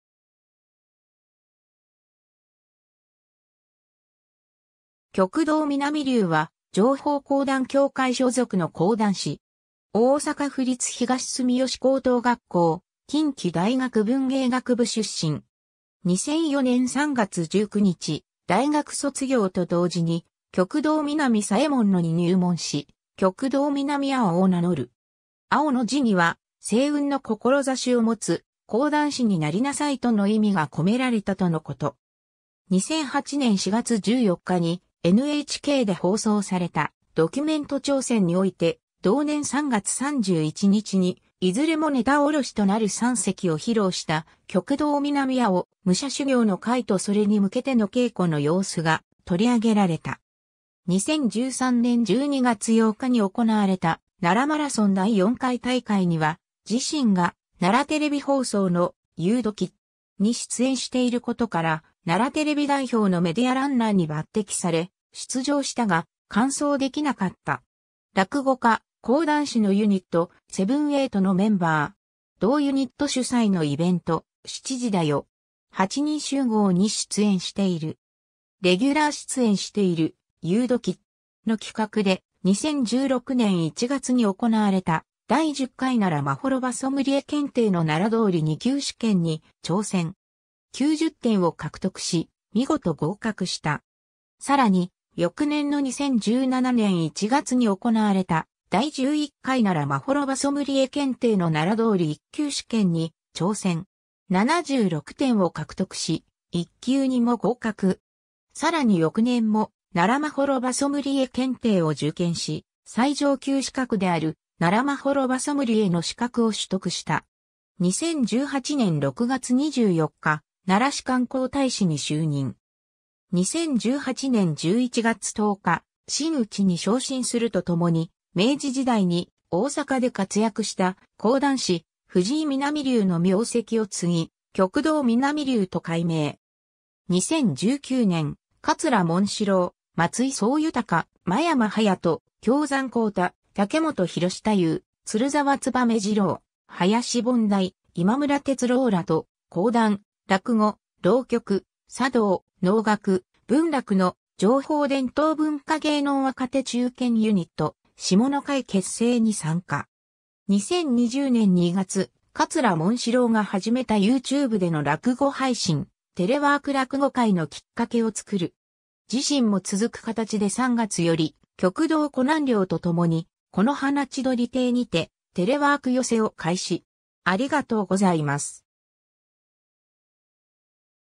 旭堂南龍
旭堂 南龍（きょくどう なんりゅう、1980年6月4日 - ）は、上方講談協会所属の講談師。
大阪府立東住吉高等学校、近畿大学文芸学部出身。
 2004年3月19日、大学卒業と同時に旭堂南左衛門に入門し、「旭堂南青」を名乗る。
「青」の字には「青雲の志を持つ講談師になりなさい」との意味が込められたとのこと。
 2008年4月14日にNHKで放送された『ドキュメント挑戦』において、同年3月31日に、いずれもネタおろしとなる三席を披露した「旭堂南青・武者修行の会」とそれに向けての稽古の様子が取り上げられた。
 2013年12月8日に行われた奈良マラソン第4回大会には、自身が奈良テレビ放送の『ゆうドキッ!』に出演していることから、奈良テレビ代表のメディアランナーに抜擢され、出場したが、完走できなかった。
 落語家・講談師のユニット「セブンエイト」のメンバー。
 同ユニット主催のイベント「7時だヨ!8人集合」に出演している。
 レギュラー出演している『ゆうドキッ!』の企画で、2016年1月に行われた「第10回奈良まほろばソムリエ検定」の奈良通2級試験に挑戦。
90点を獲得し、見事合格した。
さらに、翌年の2017年1月に行われた「第11回奈良まほろばソムリエ検定」の奈良通1級試験に挑戦。
76点を獲得し、1級にも合格。
さらに翌年も奈良まほろばソムリエ検定を受験し、最上級資格である「奈良まほろばソムリエ」の資格を取得した。
 2018年6月24日、奈良市観光大使に就任。
2018年11月10日、真打に昇進するとともに、明治時代に大阪で活躍した講談師・藤井南龍の名跡を継ぎ、「旭堂南龍」と改名。
 2019年、桂紋四郎・松井宗豊・真山隼人・京山幸太・竹本碩太夫・鶴澤燕二郎・林本大・今村哲朗らと講談・落語・浪曲・茶道・能楽・文楽の上方伝統文化芸能若手中堅ユニット「霜乃会」結成に参加。
 2020年2月、桂紋四郎が始めたYoutubeでの落語配信「テレワーク落語会」のきっかけを作る。
自身も続く形で3月より、旭堂小南陵とともに此花千鳥亭にて「テレワーク寄席」を開始。